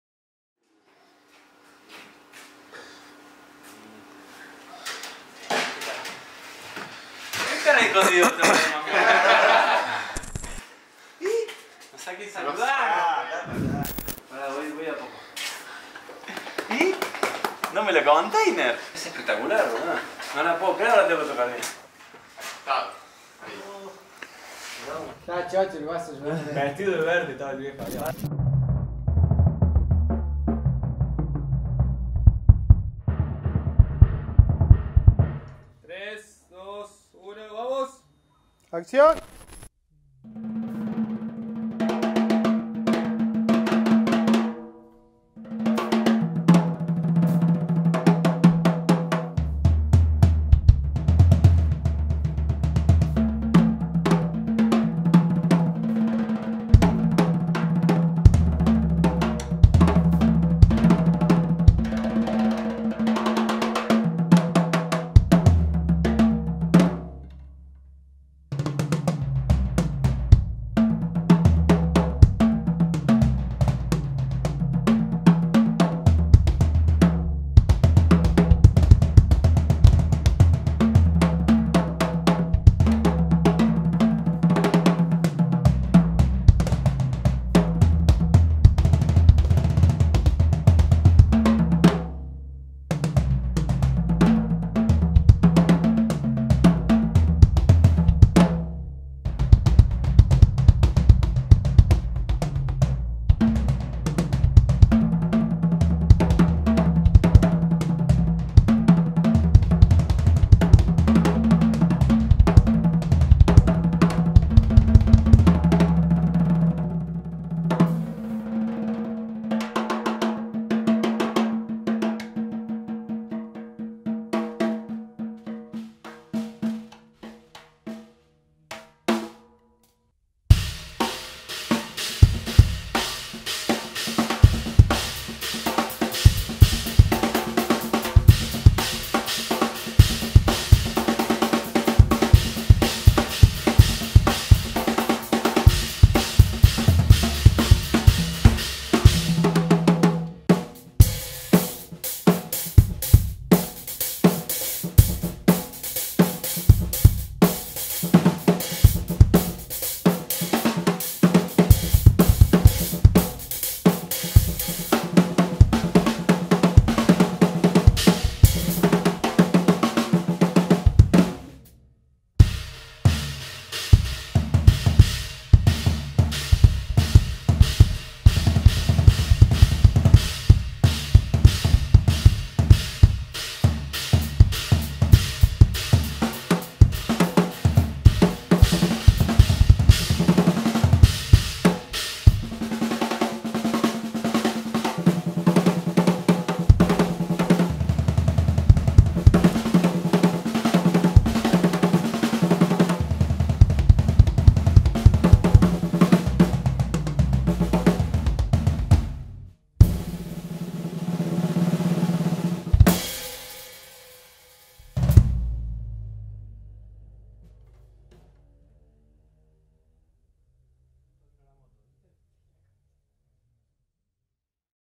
Sesiones de Taller 1 / Workshop Sessions 1, Ep 1 Andy Vilanova
Sesiones en vivo en el taller de Ocho Catorce. Andy y Bosque ,su set signature, se encuentran por primera vez! ENGLISH BELOW

Especificaciones:
Estructura: PENTA
Terminación: Raíz de Nogal con incrustaciones de ébano / Verde sólido
Medidas:
-24x12"
-20x3" Gong convertible a bombo simple tensión
-18x14", Aros de madera. Estructura de bombo adaptada a tom.
-13x7" Tom con cuerdas; Doble tirabordona.  Angulo roundover batidor, doble 45° resonante.
-10x7"

Live sessions at the Ocho Catorce workshop. Andy and Bosque, their signature set, meet for the first time!

Specifications:
Structure: PENTA
Finish: Walnut Burl with Ebony Inlays / Solid Green
Sizes:
-24x12 " Bass drum
-20x3 " Gong convertible to single tension bass drum
-18x14 ", Floor tom with wood hoops  (Bass drum structure adapted to tom)
-13x7 " Snom/Tom with strings; Double strainer. Beater roundover bearing edge, resonant double 45 °
-10x7 " Tom